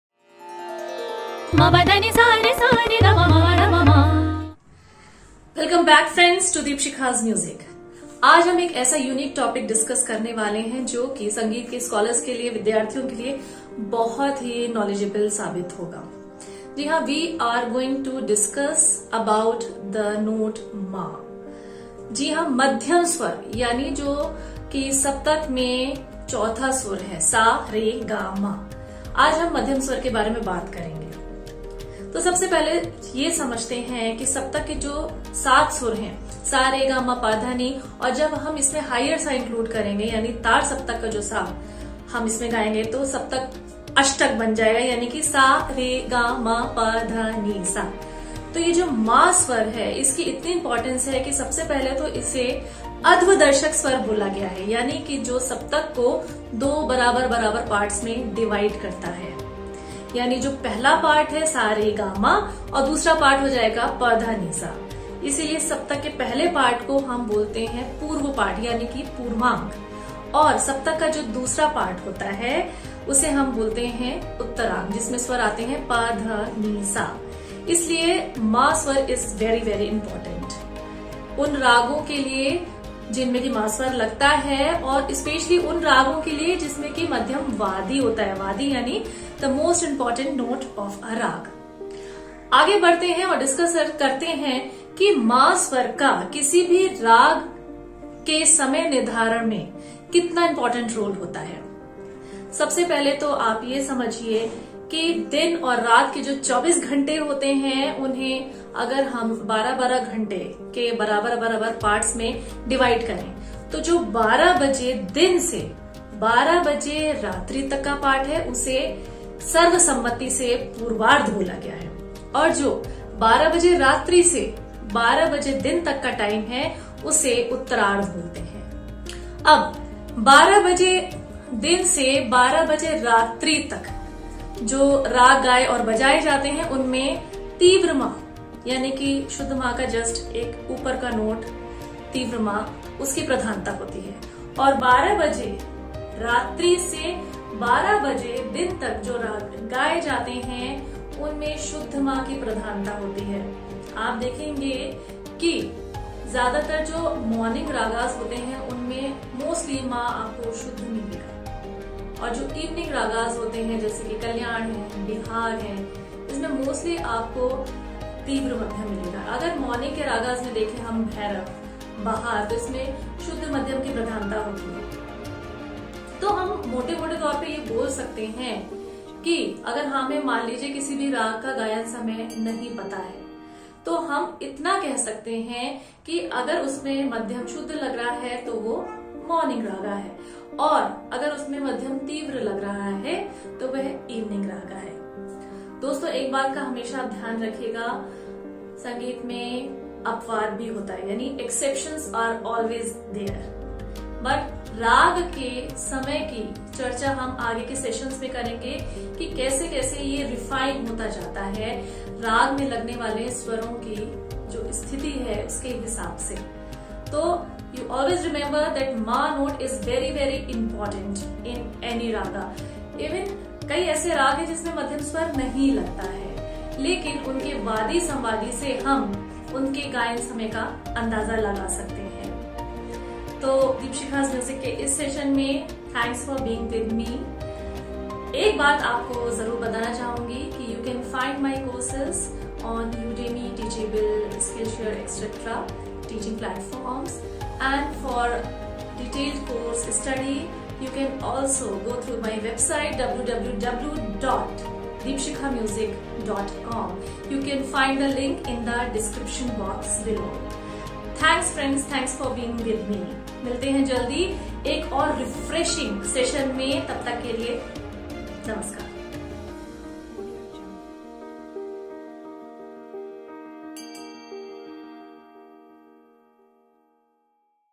Lesson 19|Importance of note Ma| Deepshikhamusic
There are seven natural notes in Indian music. Natural notes
are called 'SHUDH SVAR’. Their names are Sa, Re, Ga, Ma, Pa, Dha, Ni.
Madhyam is the fourth svara from the seven svaras of Hindustani music.

The svara of Madhyam is tivra and Shuddha. In fact Madhyam is the only tivra svara in the Saptak (Octave). It is the immediate next svara of Gandhar. "Ma" is a Shuddha Svara as well as a Tivra Svara but Ma is the only Tivra Svara out of the seven svaras.

Note:
 Ragas from Kalyan Thaat, Poorvi Thaat, Marwa Thaat, Todi Thaat have Tivra Madhyam, rest of the thaats have Shuddha Madhyam.

 Ragas where Ma is the Vadi svara - Raga Kedar, etc.

 Ragas where Ma is the Samvadi svara - Raga Malkauns.

So let's understand more about the Svar "Ma" in-depth...